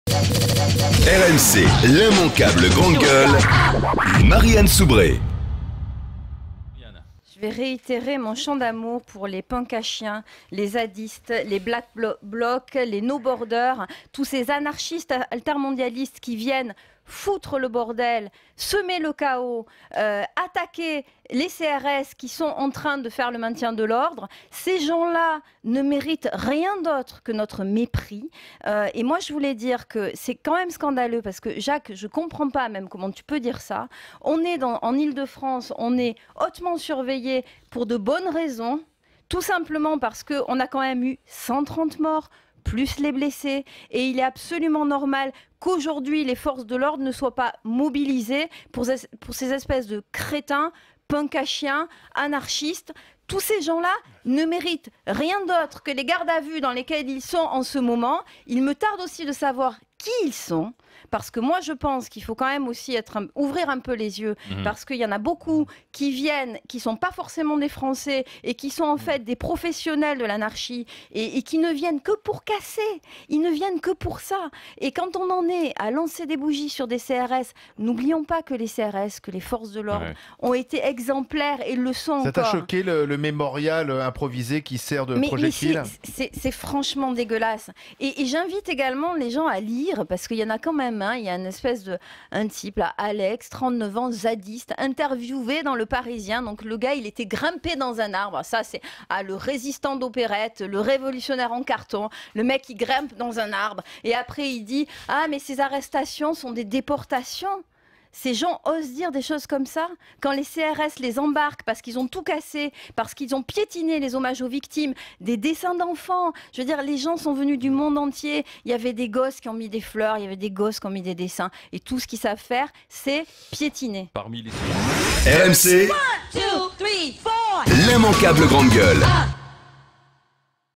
"Tous ces anarchistes ne méritent rien d'autre que notre mépris !" Marie-Anne Soubré
Hier, la place de la République fut le théâtre de violences en marge de manifestations autour de la Cop21.
Des fleurs, des bougies destinées aux victimes des attentats du 13 novembre dernier, ont été utilisées comme projectiles envers les forces de l'ordre.
Notre GG, Marie-Anne Soubré excédée, a exprimé le mépris qu'elle avait pour " ces anarchistes altermondialistes."